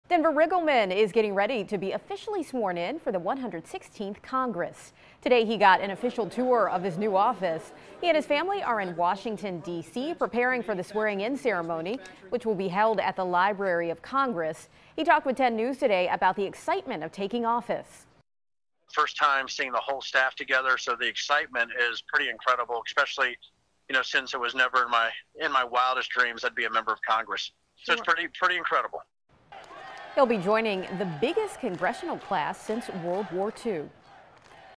Denver Riggleman prepares to be officially sworn in
He will be sworn into the 116th Congress.